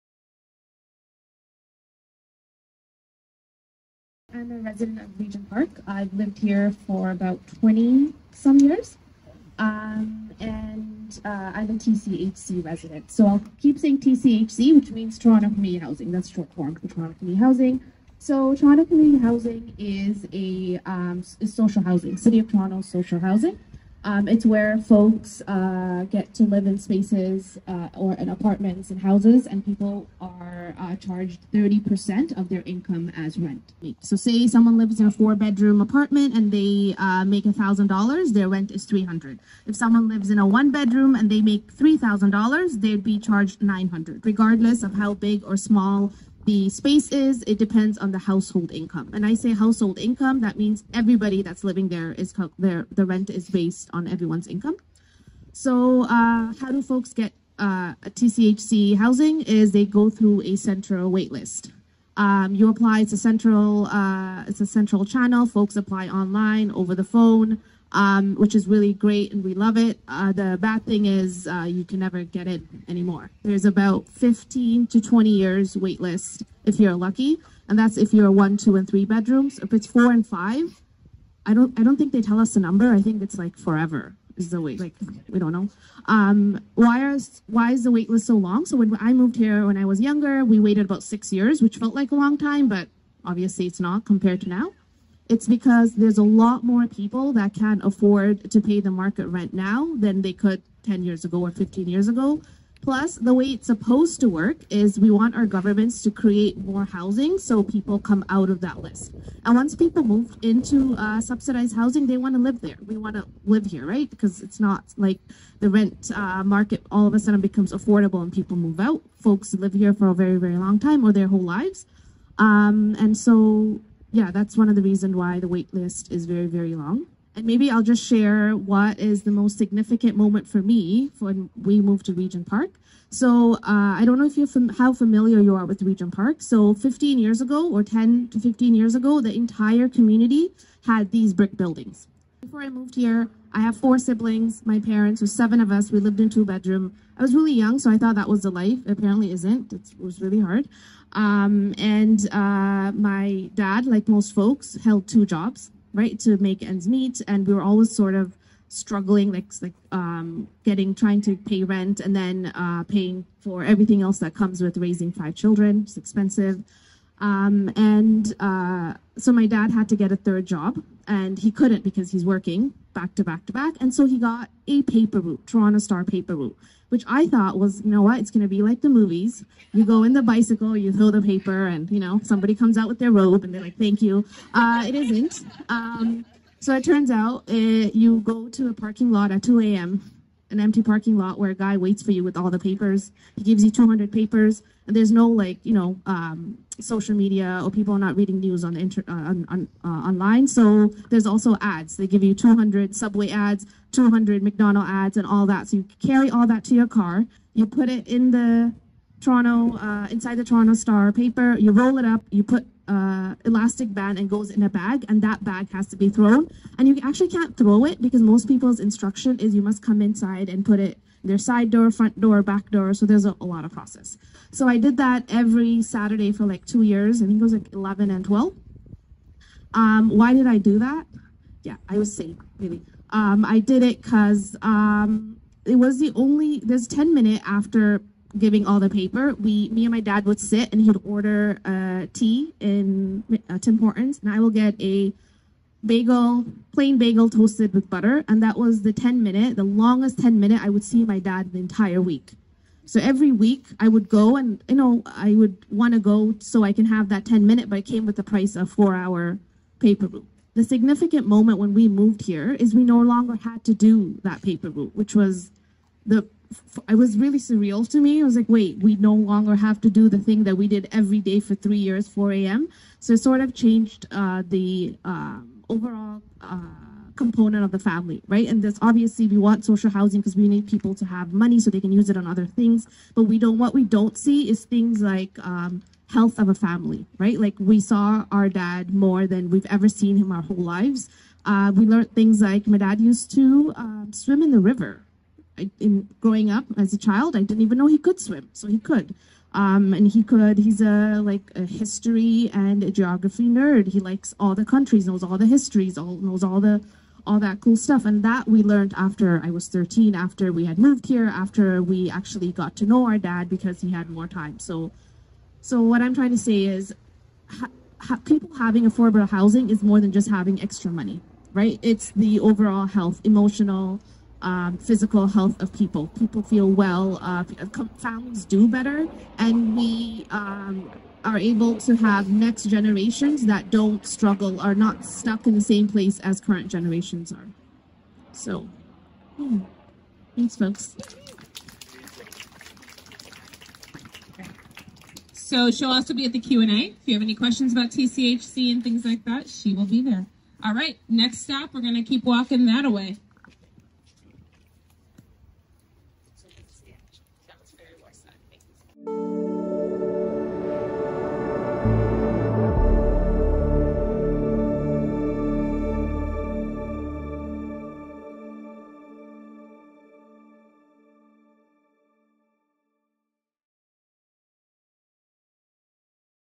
The Importance of Subsidized Housing
Maseeda Majeed speaks about her experience of growing up in Regent Park, Toronto, and the importance of subsidized housing to her family's welfare during her childhood. Maseeda grew up in Toronto Community Housing provided in Regent Park. She spoke as part of the How We House Regent Park Tour in the Fall of 2024.